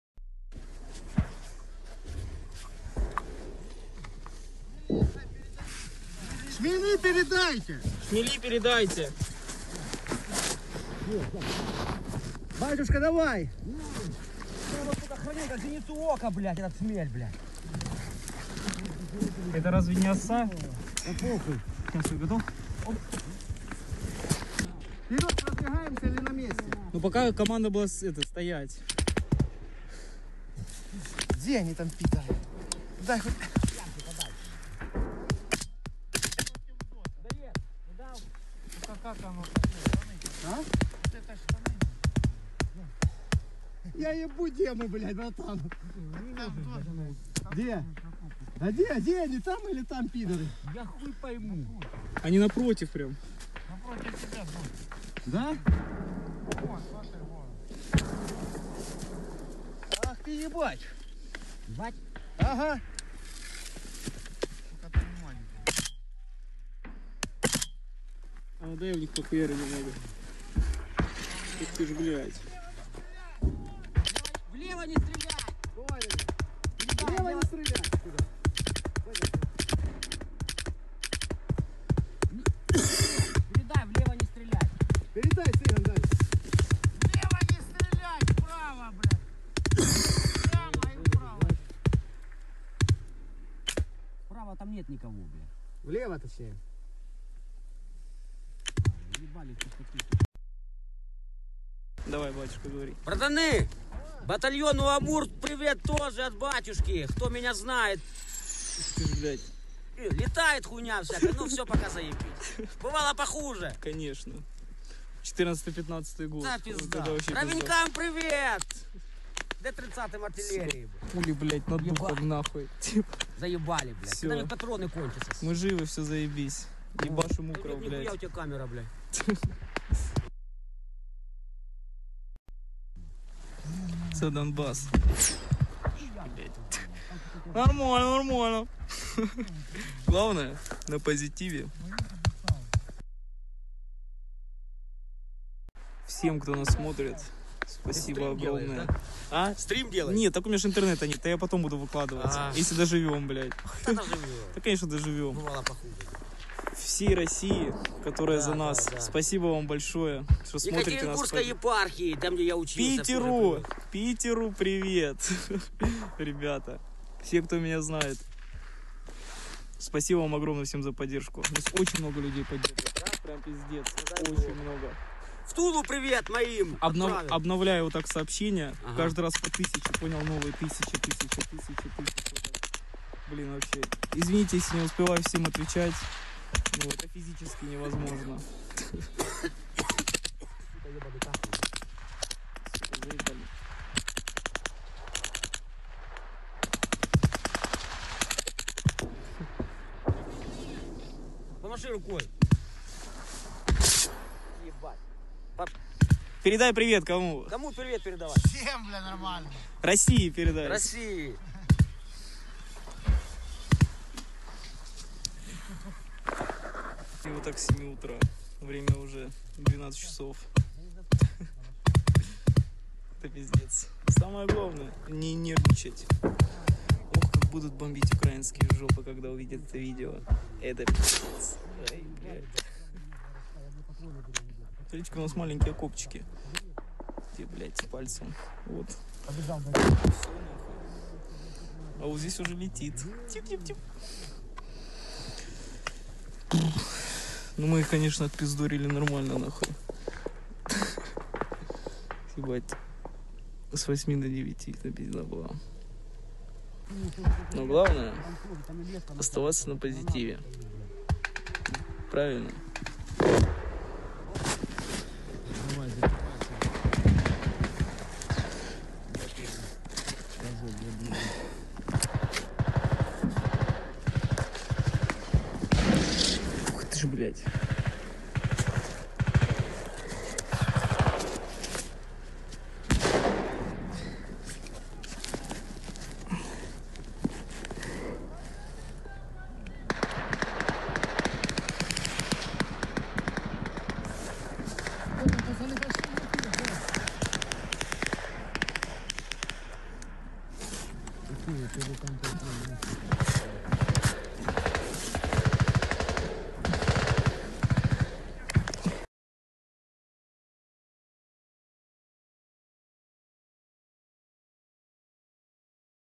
Ukraine War - War Militia DNR. Soldiers on the front lines. War mood!
Since 2014, they have been called separatists, agents of the Kremlin and the Russian Army.